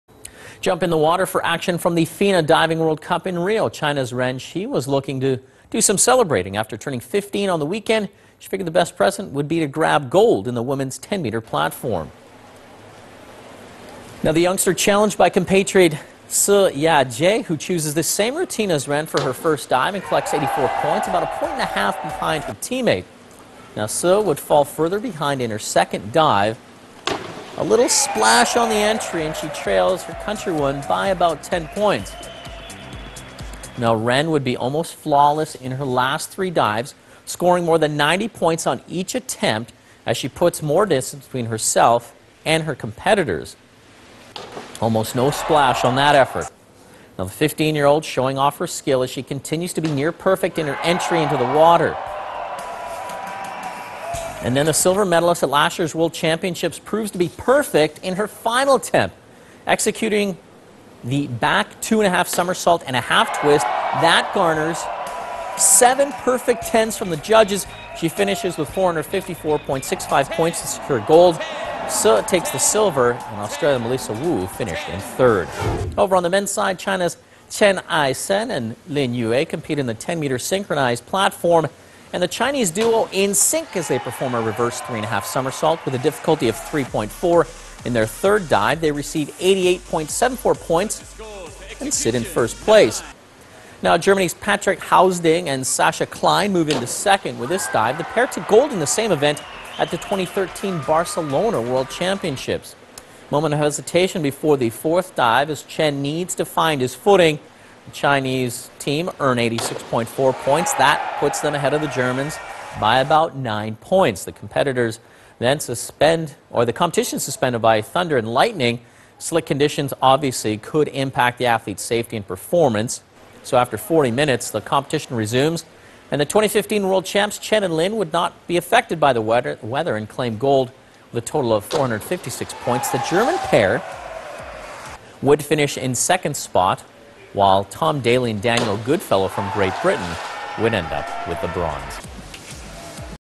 Chinese competitor celebrates 15th birthday with gold medal
FINA DIVING WORLD CUP - RIO